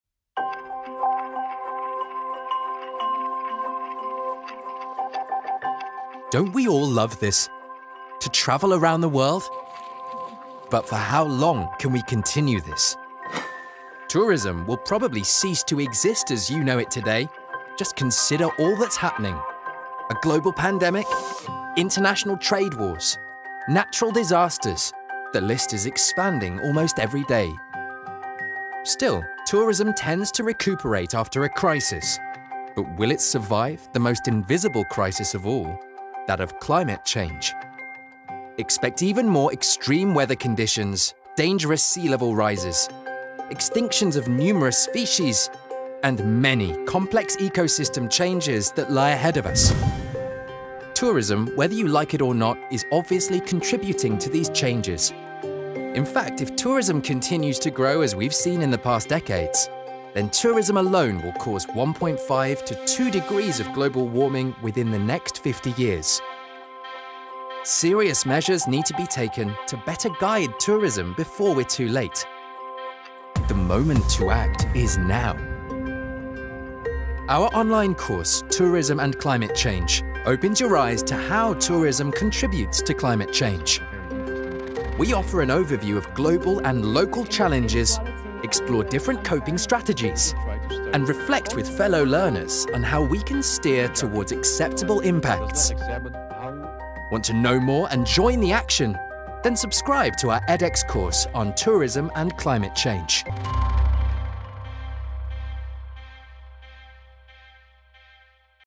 Tourism and Climate Change | WUR MOOC on EdX
Don’t we all love to travel the world? But for how long can we continue this in the way we know, is unsure.
Tourism will probably cease existing the way it was considering all that is happening.

A global pandemic, financial crises, sea level rise, declining biodiversity in ecosystems and more.
Tourism tends to recuperate after crises, but will it survive the most invisible crisis of all:
that of climate change?

In this free MOOC by Wageningen University and Research, you’ll learn all about the key challenges that tourism is facing in context of drastic climate change-, mitigation and adaptation. Start learning today!